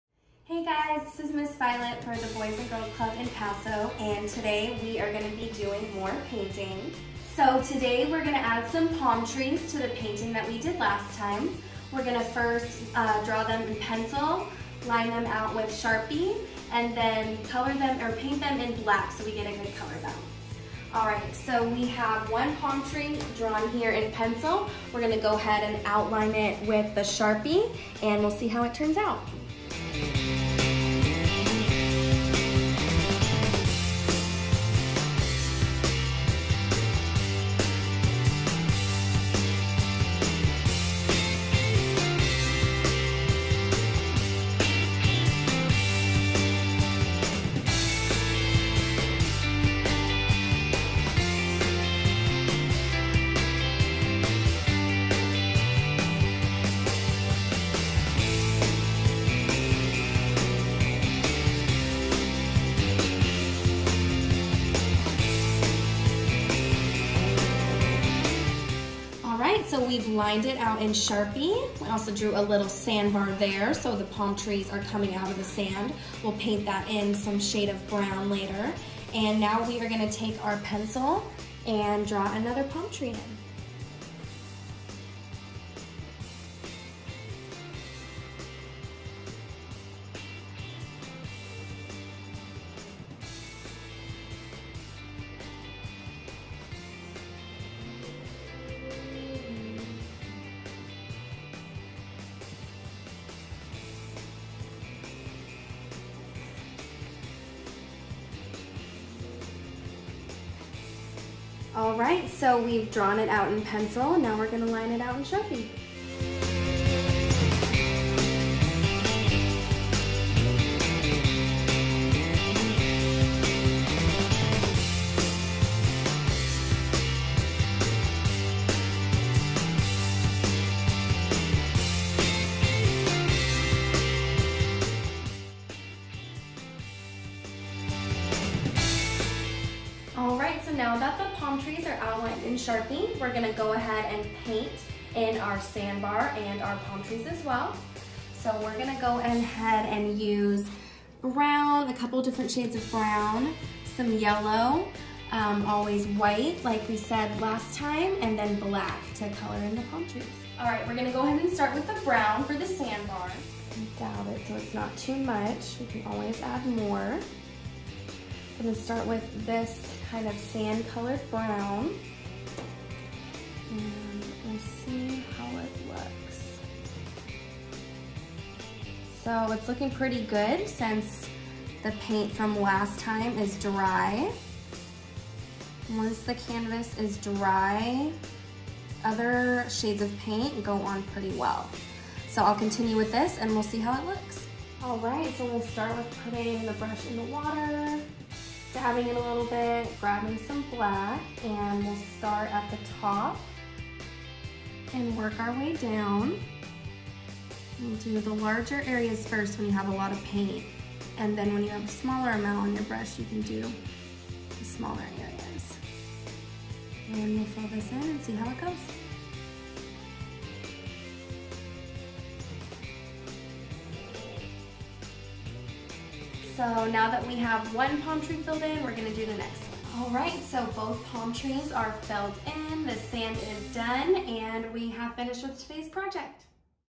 Ms. Violet Paint Blending #2
Check out Ms. Violet's second part of her paint blending video. Grab the canvas you painted yesterday, gather your supplies, and get ready to have some fun.